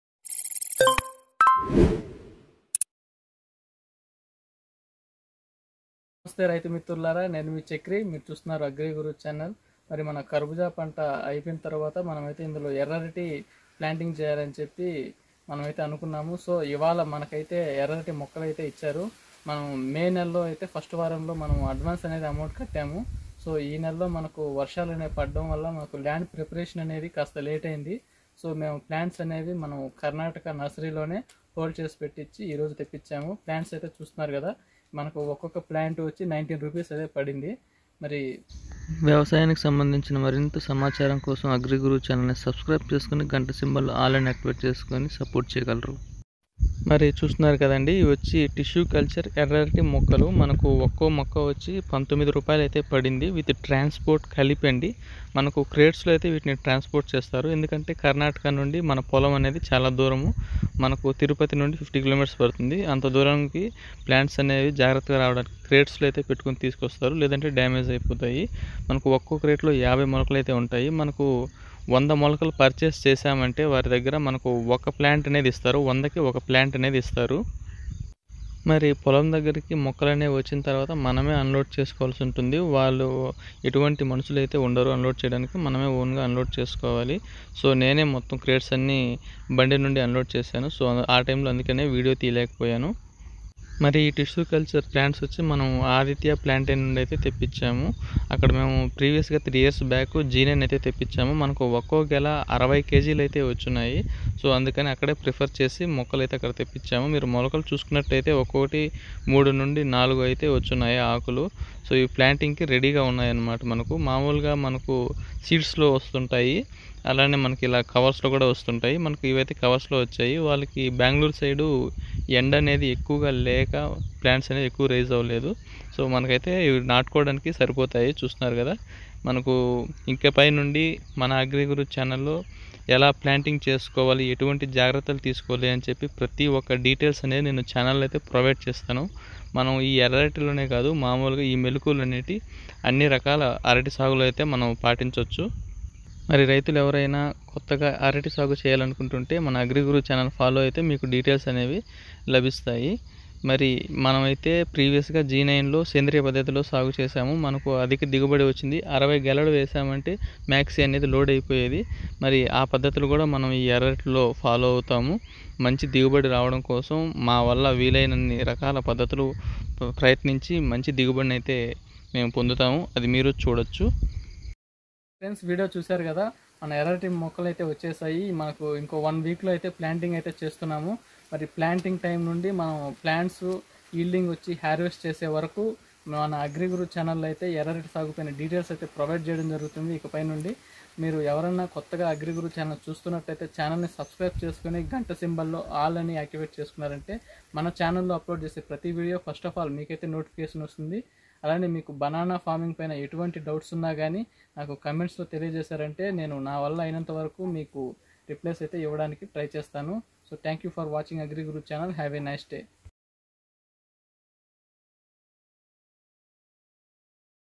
Tissue Culture Red Banana Plants మేము తీసుకున్న ఎర్ర అరటి మొక్కల వివరాలు|AGRI GURU
ఎర్ర అరటి మొక్కల వివరాలు మరియు సాగు చేసే విదానం ఇక పై మన చానెల్లో రైతులకు ఉపయోగకరంగా ఉంటుందని అందించడం
 జరుగుతుంధీ

Tissue లేదా అరటి మొద్దుల కొరకు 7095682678 whatsapp ద్వారా సంప్రదించగలరు